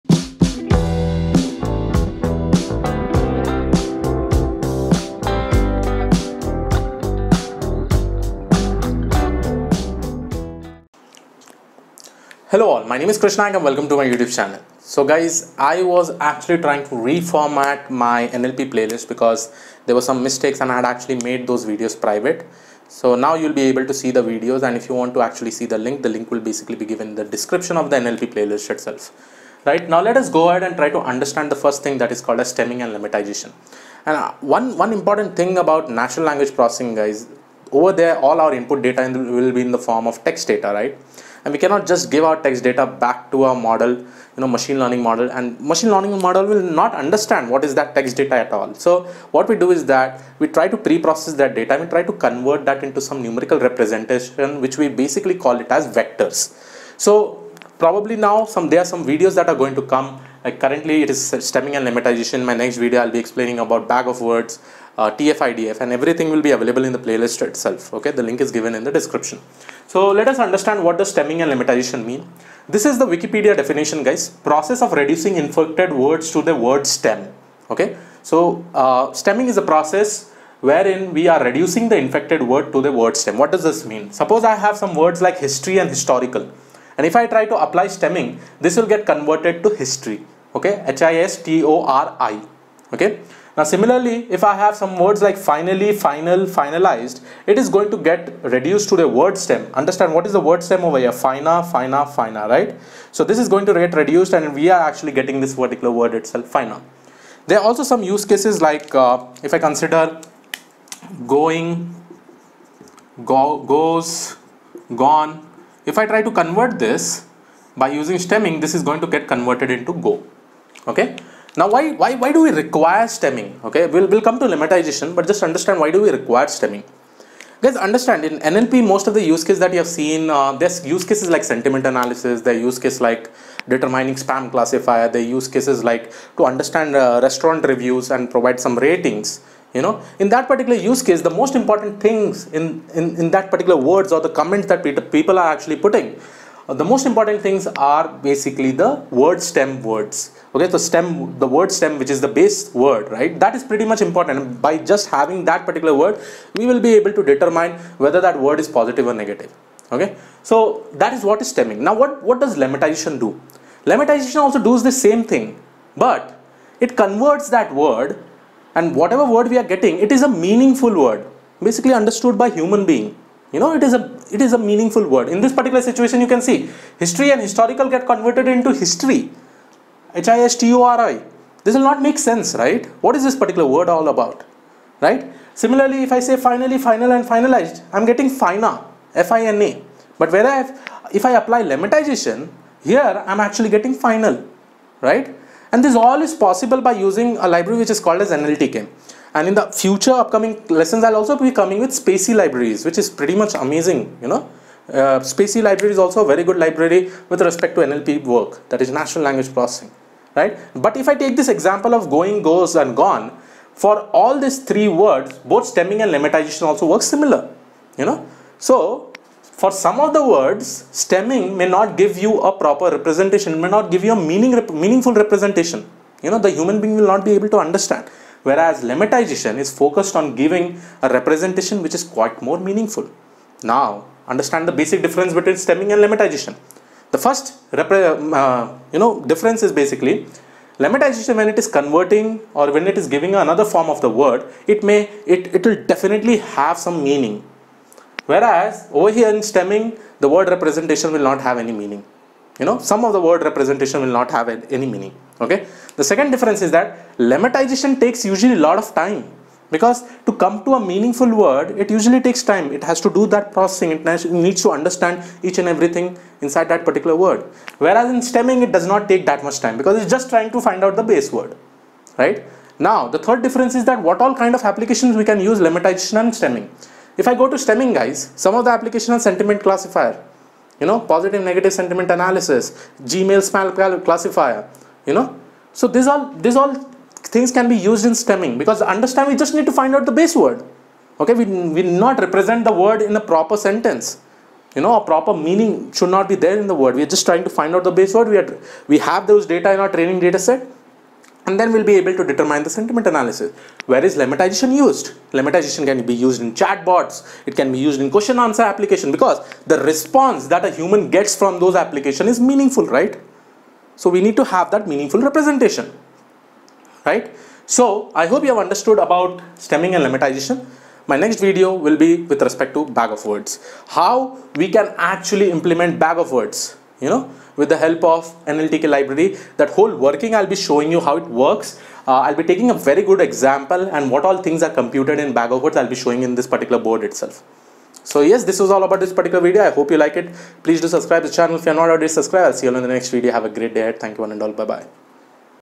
Natural Language Processing| Stemming And Lemmatization Intuition
In this video we will understand the detailed explanation of Lemmatization and understand how it can be used in Natural Language Processing. We will also see the basic difference between Lemmatization and stemming.

GPay: krishnaik06@okicici